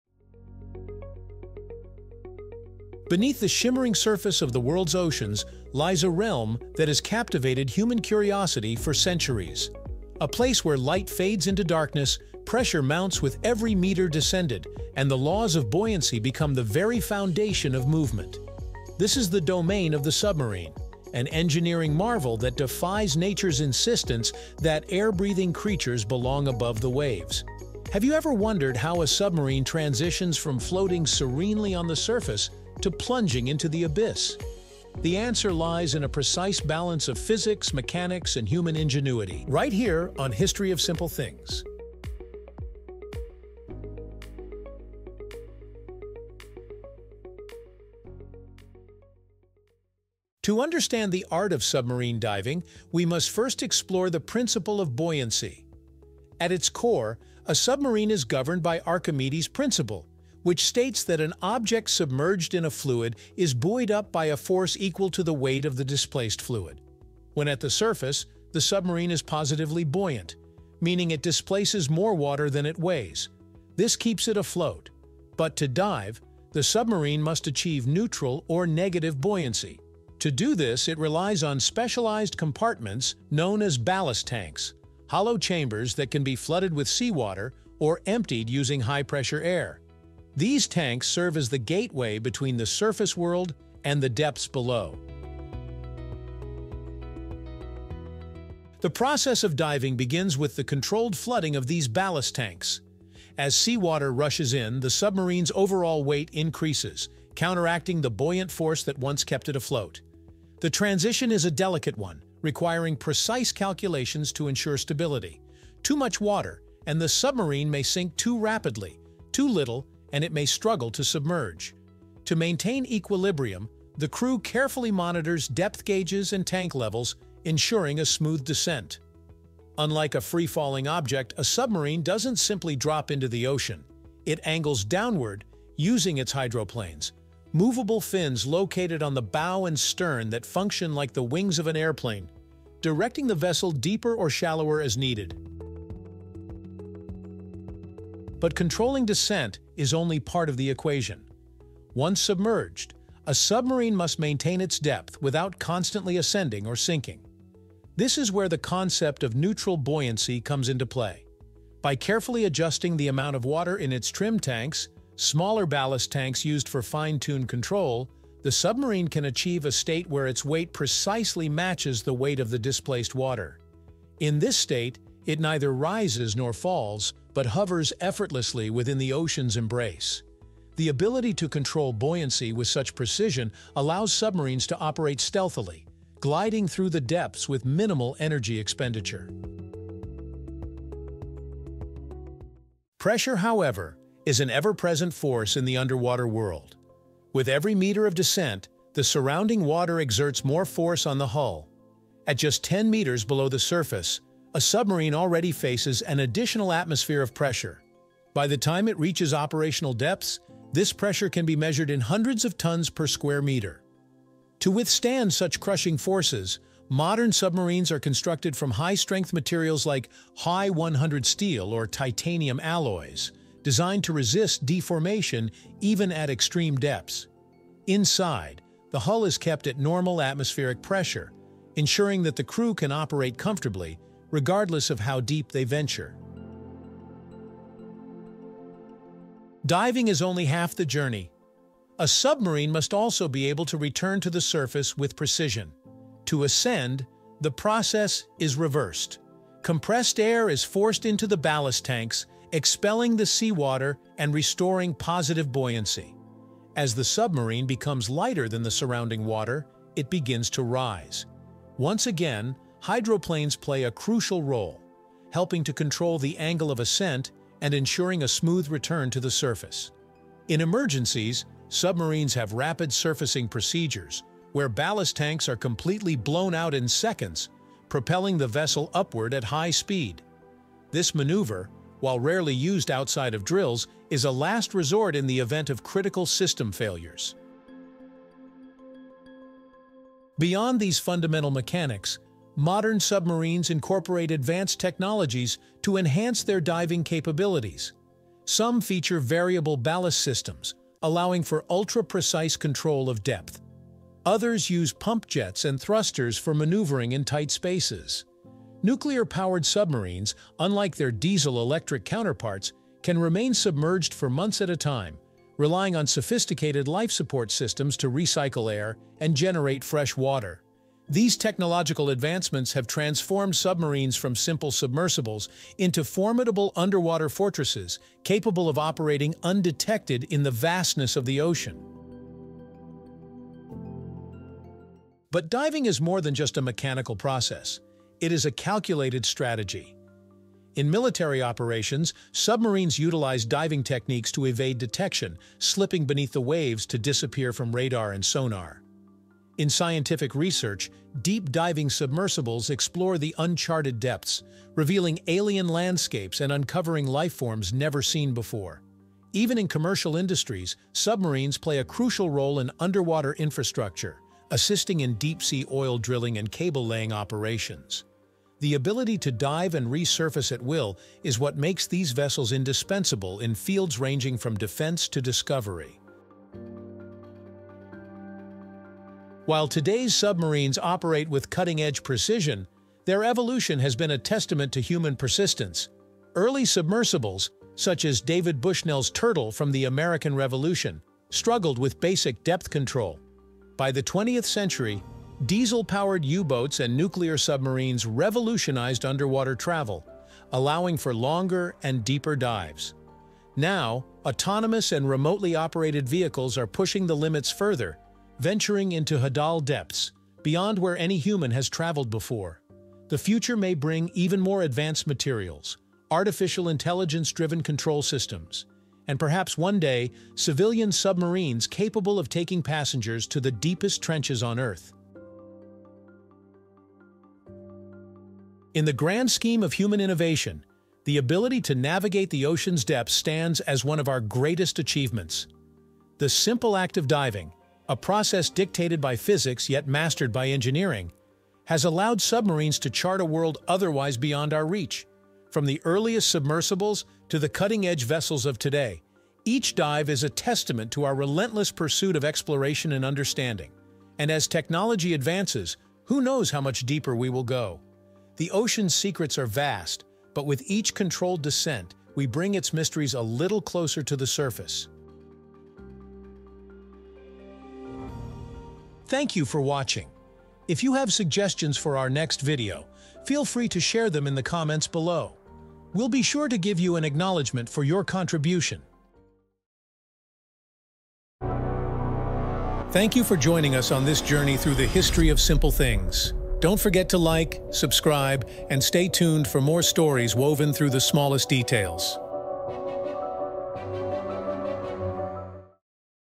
How Do Submarines Sink Without Sinking?
Beneath the waves lies a world of pressure, darkness, and precision engineering—welcome to the incredible realm of submarines. Have you ever wondered how these vessels dive deep into the ocean and resurface with ease? The answer lies in the perfect balance of physics, mechanics, and human ingenuity.

Join us on History of Simple Things as we explore the science and technology behind submarine diving!

Timestamps:
0:00 - Introduction: The Realm of Submarines
0:49 - The Principle of Buoyancy
1:37 - How Submarines Dive
2:27 - Maintaining Depth and Neutral Buoyancy
3:11 - The Challenge of Water Pressure
3:58 - Returning to the Surface
4:49 - Advanced Technologies in Modern Submarines
5:36 - Strategic and Industrial Applications
6:18 - The Evolution of Submarines
7:07 - The Future of Deep-Sea Exploration
7:48 - Conclusion: The Pursuit of the Deep